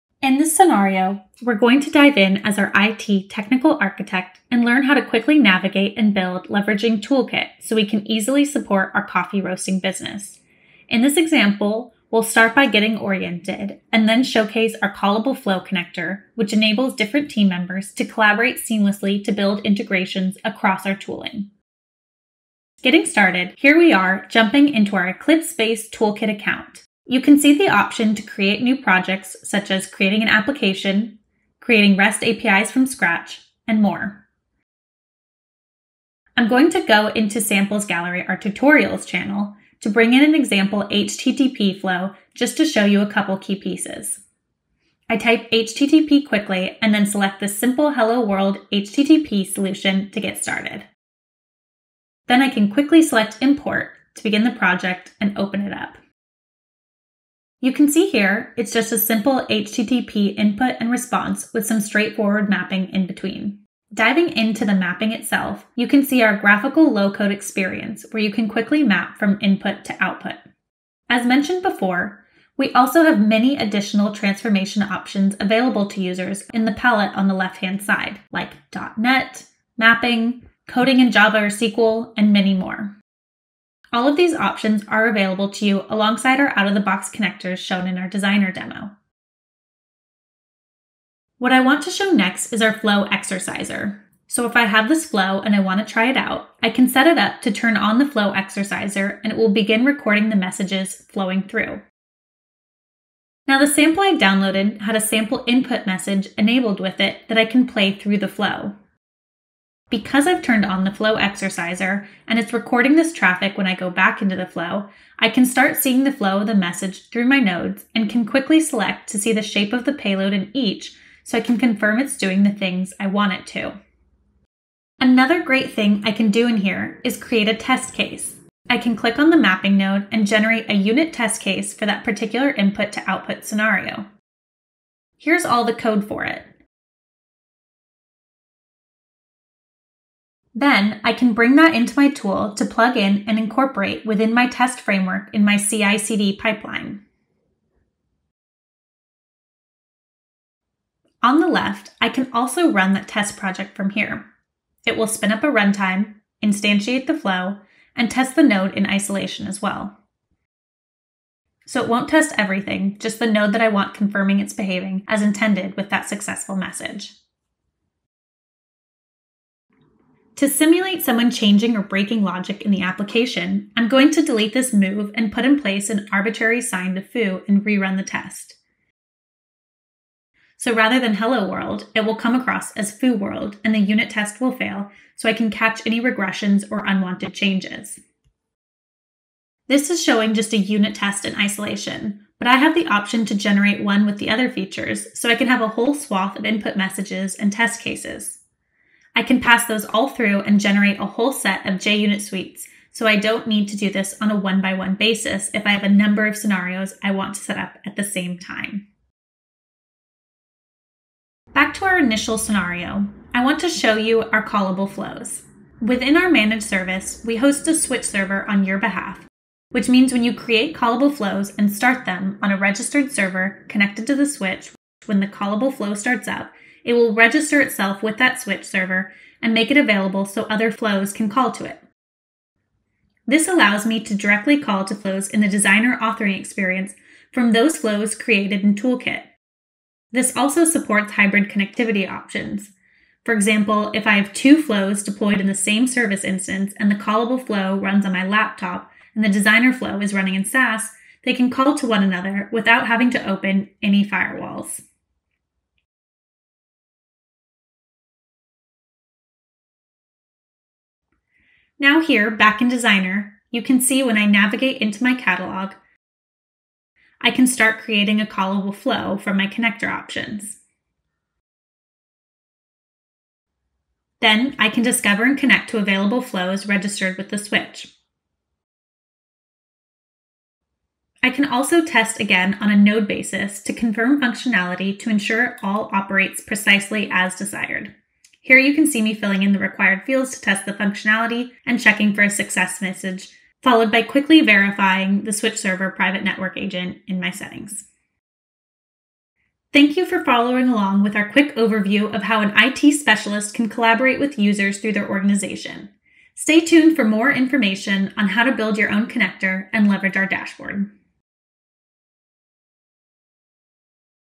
How to use the Toolkit - IBM App Connect
In this scenario, we demonstrate how to quickly navigate and build using the Toolkit. We also showcase our callable flow connector which enables different team members to collaborate seamlessly to build integrations across our tooling.